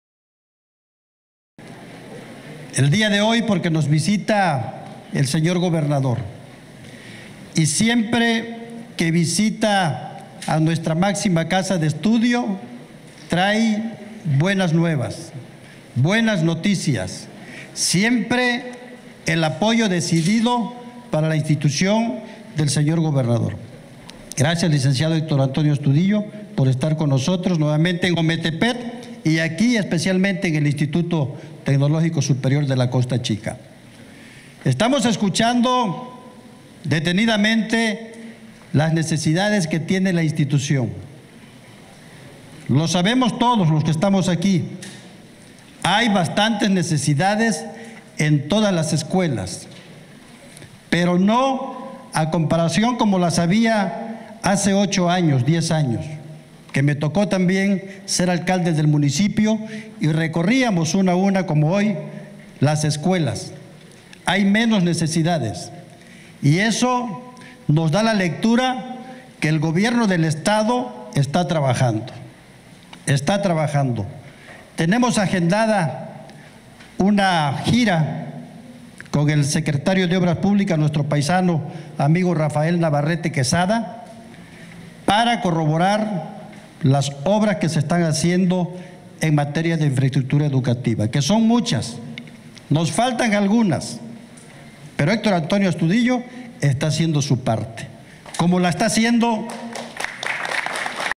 GCP EFRÉN ADAME PDTE OMETEPEC TECNOLÓGICO COSTA CHICA
El gobernador  Héctor Astudillo atendió con alumnos del Instituto Tecnológico Superior de la Costa Chica

Ometepec, Gro.- Con el fin de mantener un  diálogo directo con todos los sectores de la población y en cumplimiento a su compromiso de un encuentro con la comunidad estudiantil, el gobernador Héctor  Astudillo Flores visitó las instalaciones del Instituto Tecnológico Superior Costa Chica, ubicado en Ometepec.

En este encuentro, Astudillo Flores conoció de viva voz las necesidades de alumnos y personal docente, con quienes de manera conjunta acordaron las obras a realizar, que permitan mejorar las instalaciones físicas de la institución y coadyuvar a que su preparación académica sea en mejores condiciones.

Asimismo, el mandatario estatal se comprometió de equipar los laboratorios, impermeabilizar las aulas y dar mantenimiento a los sanitarios, así como entregar equipos de cómputo e impresoras láser, exhortando a los jóvenes a continuar con su preparación académica, refrendando el compromiso que del gobierno que encabeza para apoyar este plantel educativo.

Cabe destacar que a este plantel acuden más de mil 700 estudiantes de diferentes municipios indígenas de Guerrero y Oaxaca, quienes cursan estudios en Ingeniería y Contaduría, hecho que sido priorizado, desde el inicio de la administración de Astudillo Flores, con apoyos a este plantel, como la entrega de 2 edificios, 1 autobús y equipamiento.

En este encuentro, el Ejecutivo estatal entregó nombramiento a Juan Fermín Zúñiga Trujillo como Director General del Instituto Tecnológico Superior de la Costa Chica, quien cuenta con una amplia experiencia en el sector educativo, quien se comprometió a trabajar junto con la comunidad estudiantil para consolidar a esta institución como una de las mejores en su tipo.